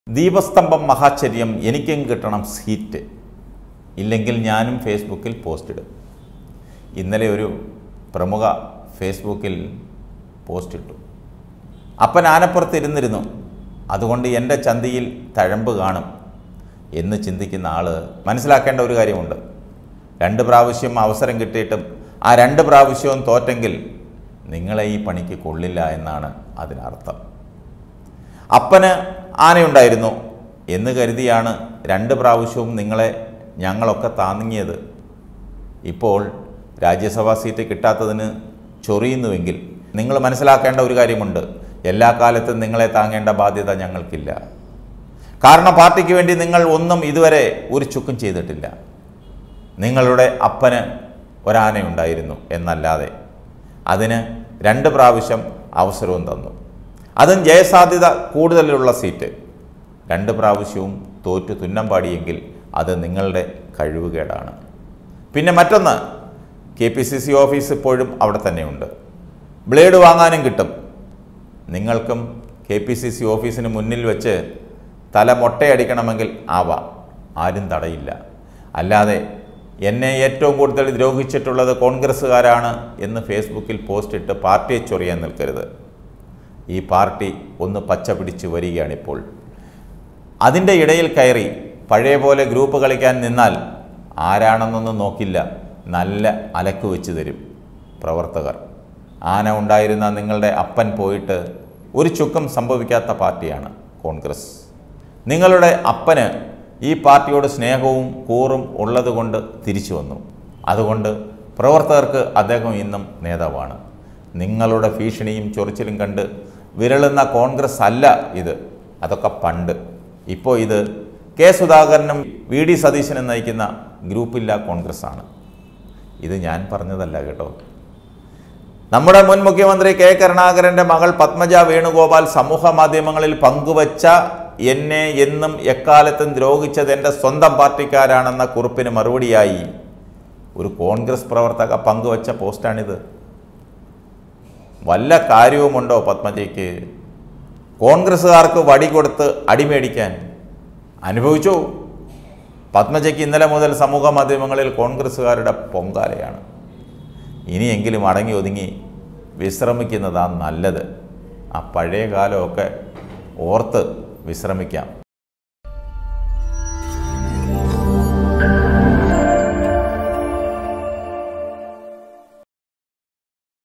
ദീപസ്തംഭം മഹാശ്ചര്യം എനിക്കും കിട്ടണം സീറ്റ് ; പദ്‌മജക്ക് കോൺഗ്രസ്സുകാരുടെ പൊങ്കാല
Tag-Generator-Youtube-malayalam news-global